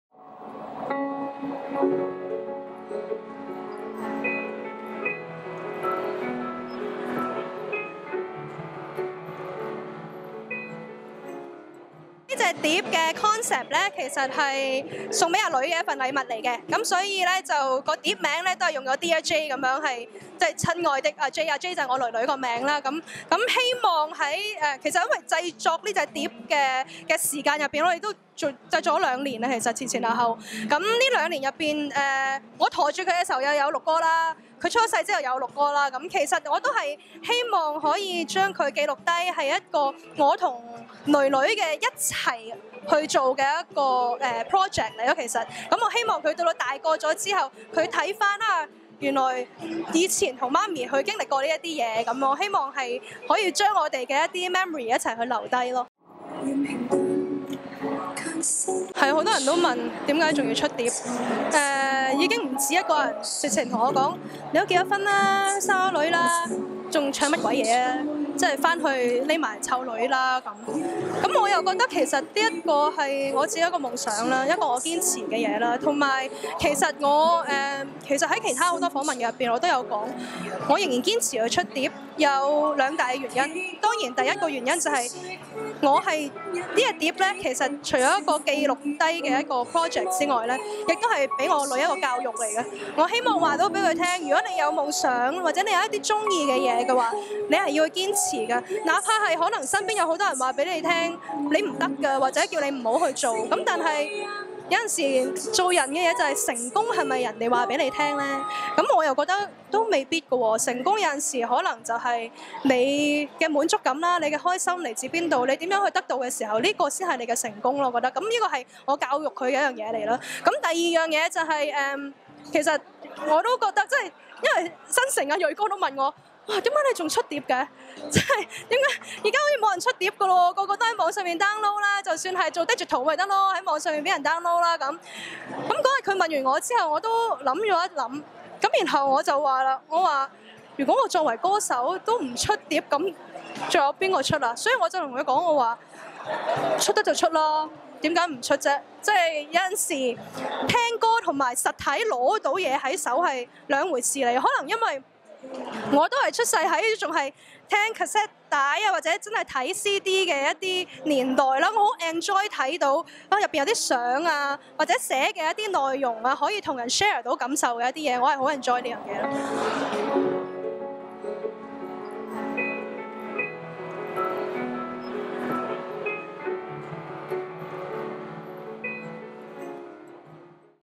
MamTv星級追蹤－【鄧芷茵June 新碟發佈】
鄧芷茵June 新碟發佈細說出這張碟的概念？在繁忙的媽媽工作同時又要堅持歌唱事業有何困難之處？